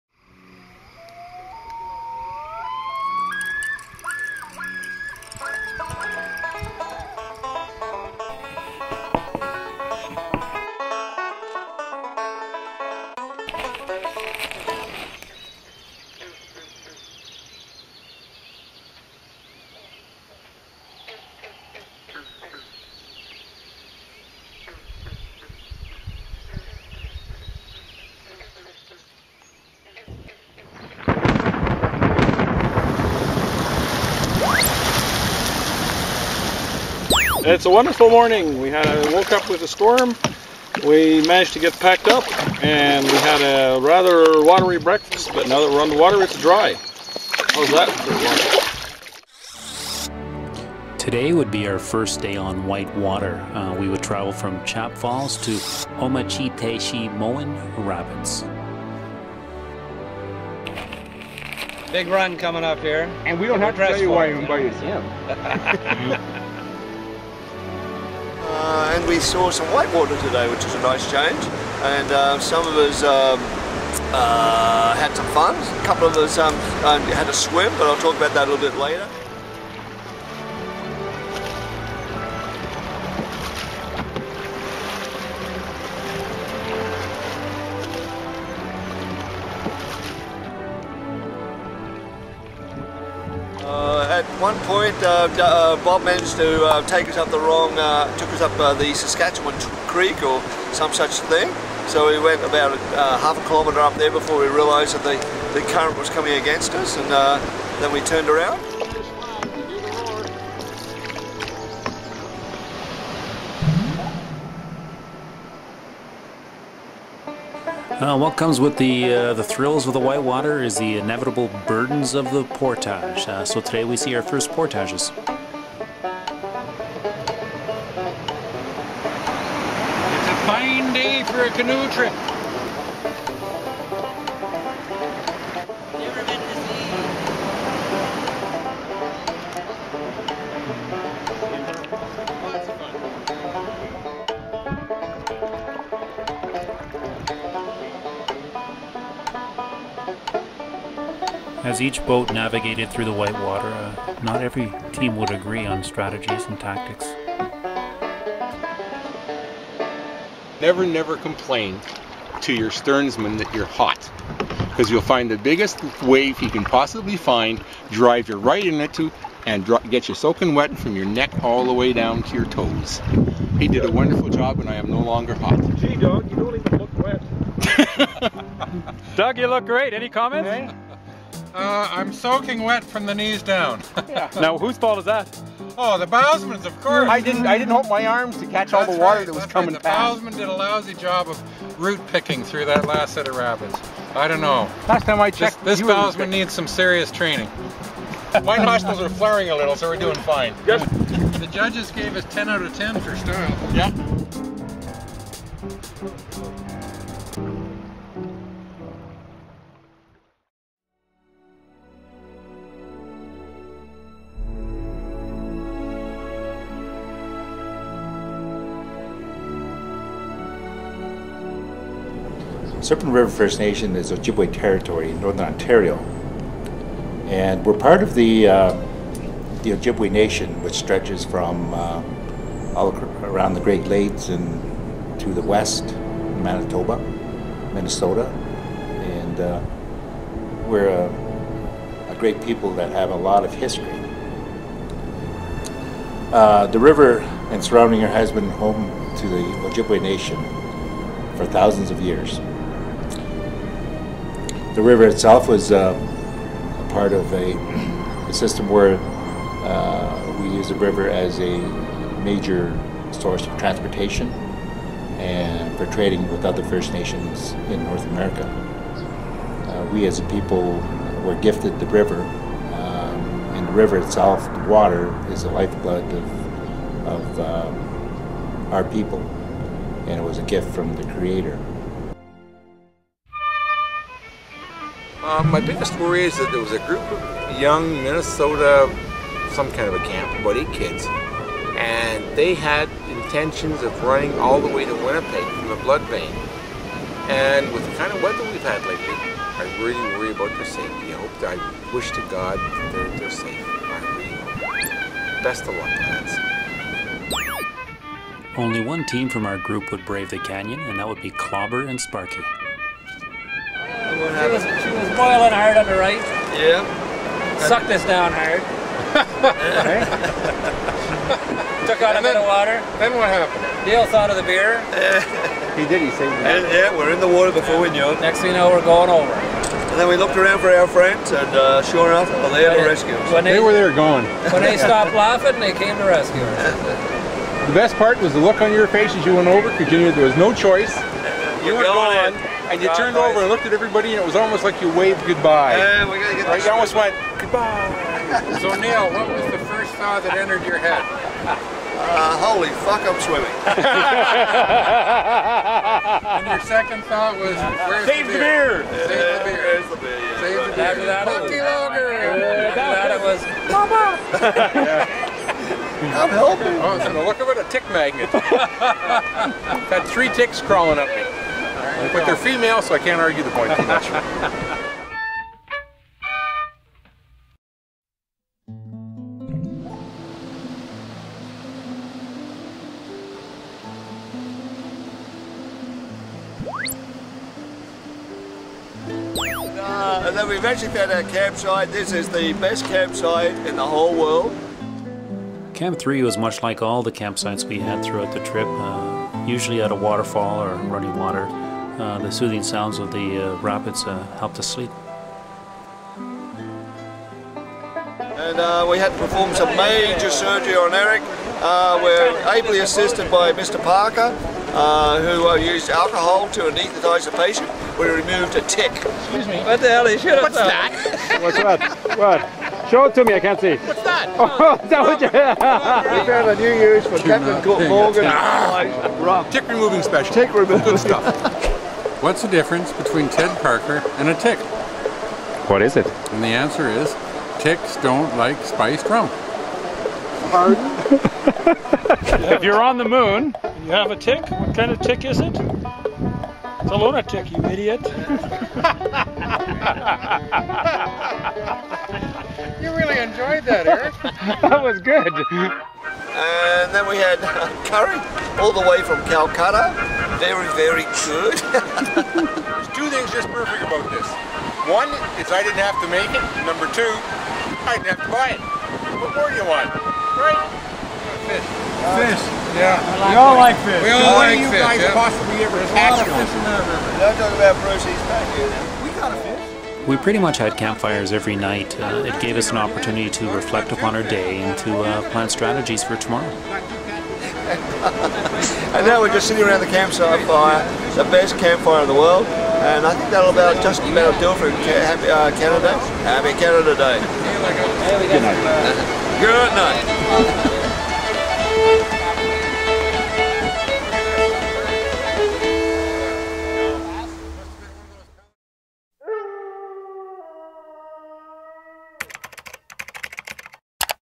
Bloodvein River Adventure - Day 3
Paddle the Great Canadian Heritage BLOODVEIN RIVER with the BRAT (Briargreen River Adventure Team). They are a group of hearty middle aged men with more enthusiasm than good sense.

This is Day 3 of 9 ...watch all 9 videos and digitally join us on our trip... and save thousands of dollars.

We are never too serious...so please don't take the videos too serious!

They are all filmed by one person, with one camera and edited on one computer once the video is back in a warm, dry studio.